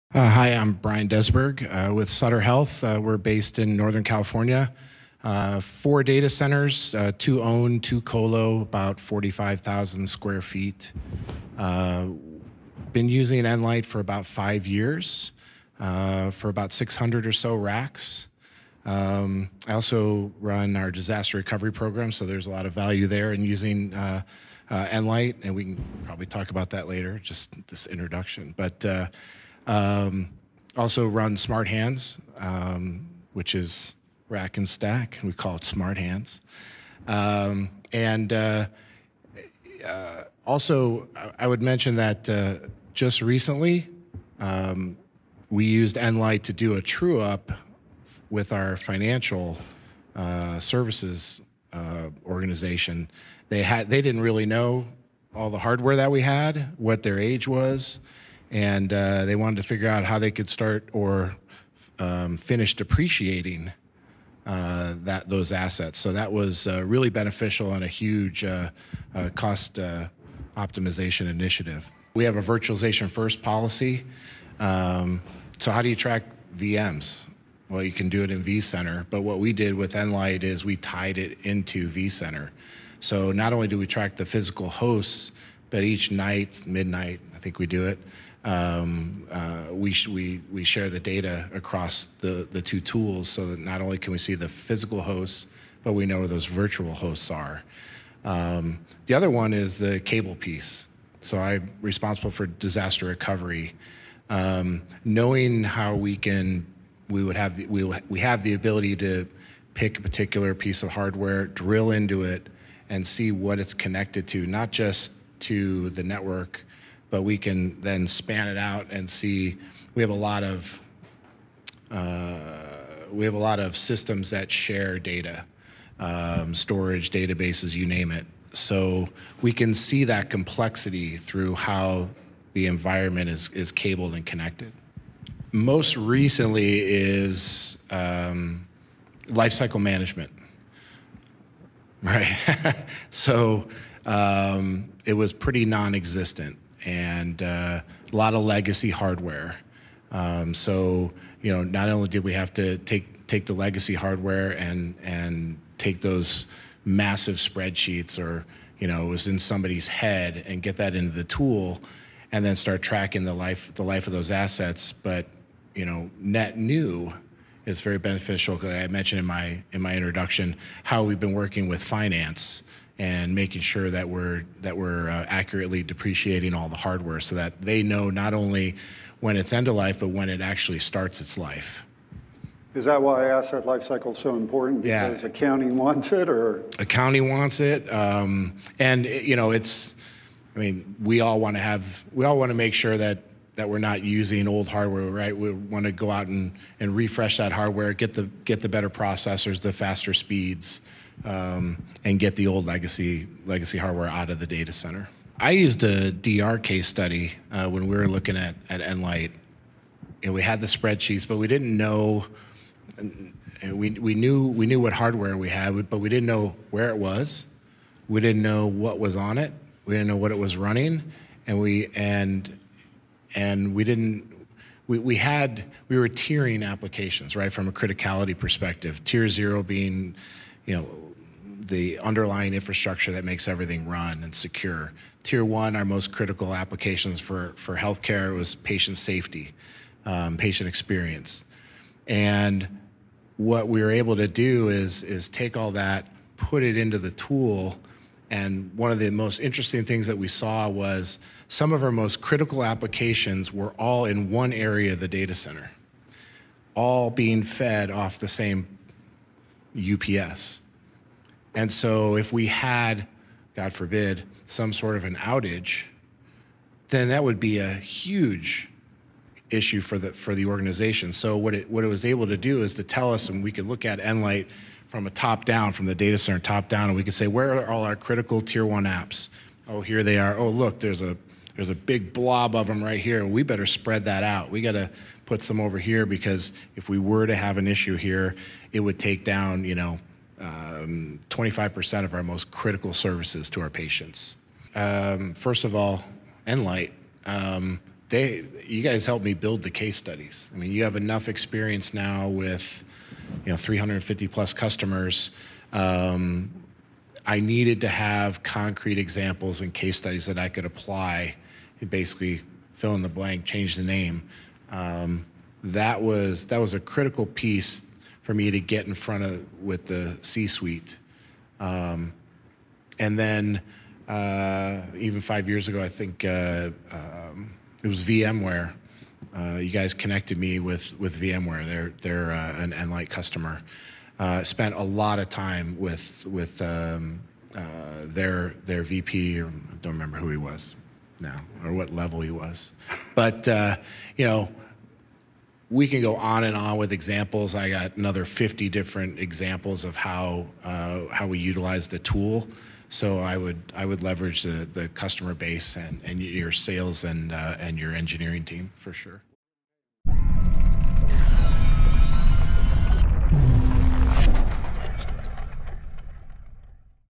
Sutter Health, Customer Testimonial, Gartner 2019
Listen to how Sutter Health resolved discrepancies between Asset management and finance systems with Nlyte Software.  Asset Reconciliation, Disaster Recovery, colocation,  cost optimization, vm management, cable management, network connectivity mapping, technology refresh.